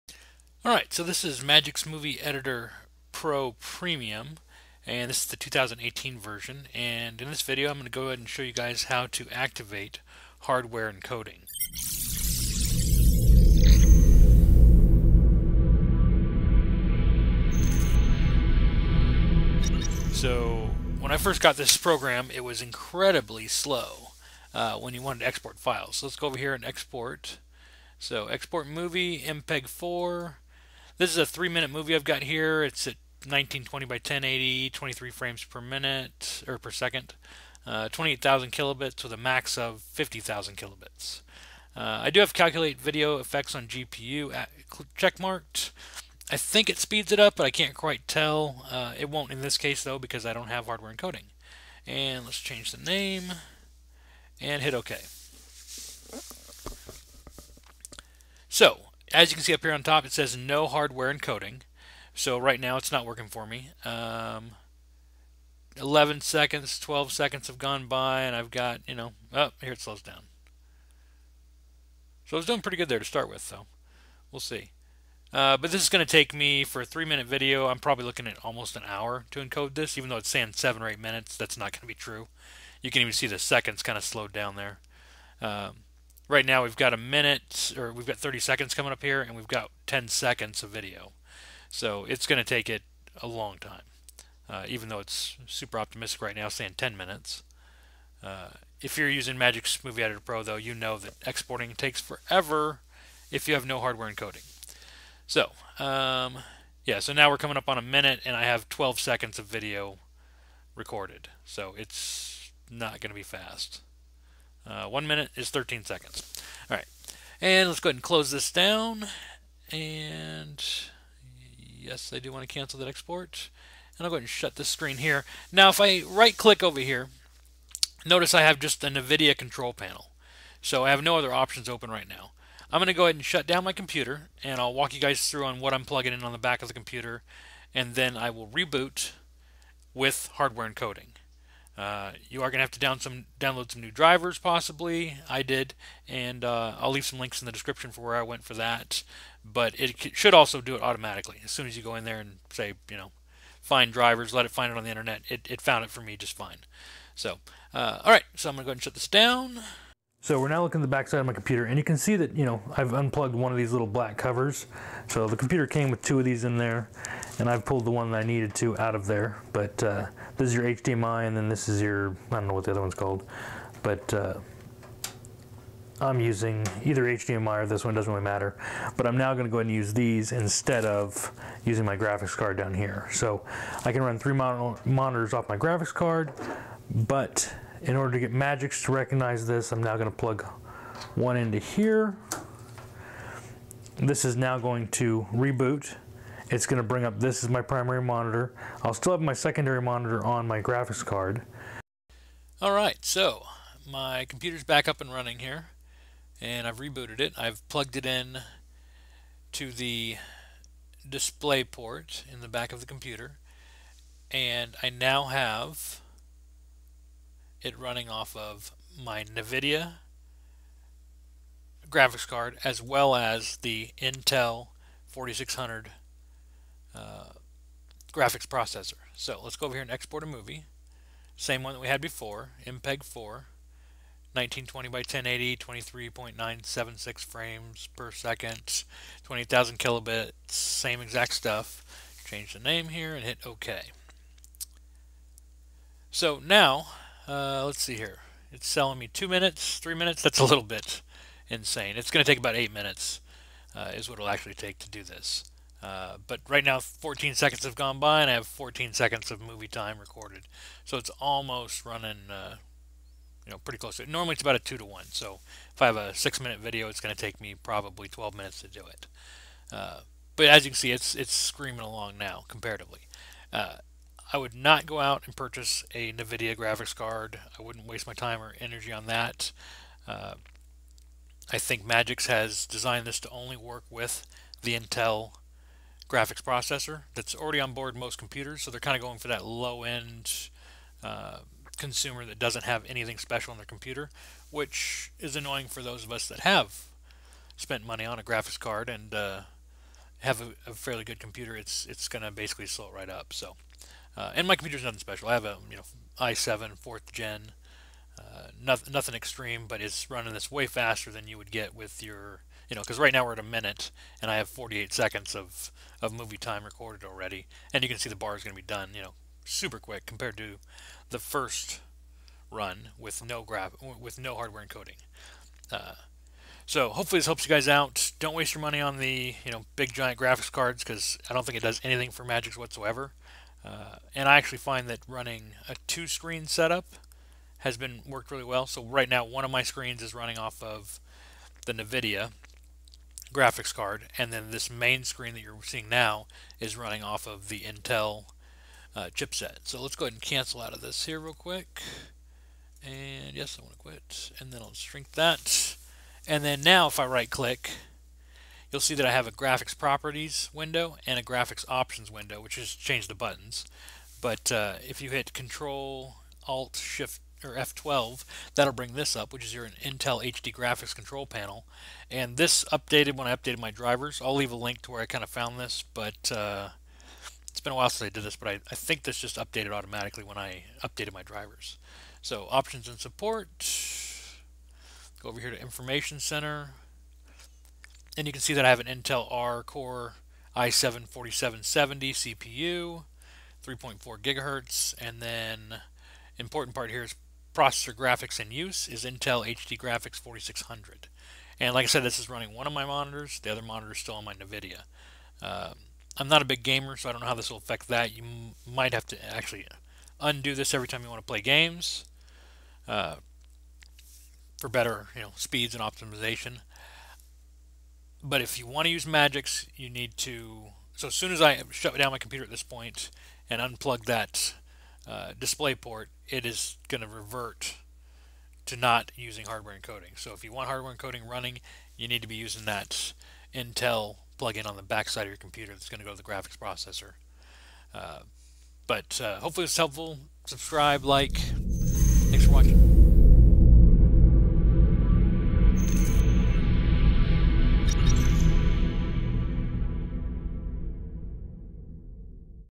MAGIX HARDWARE ENCODING HACK | HOW TO | Magix Movie Edit Pro (2019)
In this video, I explain how you can enable "Hardware Encoding" if you have an Intel processor (i7)  and why buying a graphics card is not the answer when using Magix.

I will explain how to enable hardware encoding in Magix Movie Edit Pro 2019 & 2018. I believe this hack will work with the 2017, 2016, and possibly previous versions. I was struggling with slow video export speeds when I first tried Magix Movie Edit Pro and " No hardware acceleration".

Having no hardware acceleration was driving me nuts and making 4k video editing impossible! Even a standard quality YouTube video was an all-day process to edit and export.

My Camera / Flight Bag